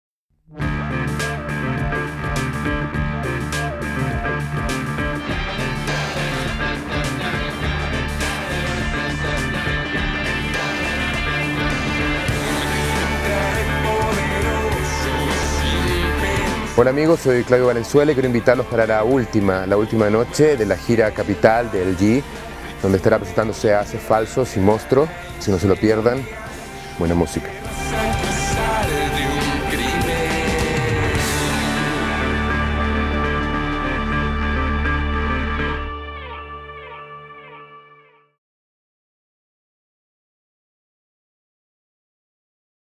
Invitación a la última fecha de LG Gira Capital
El líder de Lucybell Claudio Valenzuela nos invita a la última fecha de LG Gira Capital, donde se presentarán Ases falsos y Mostro. ¡No te lo pierdas!

¿Quieres ir?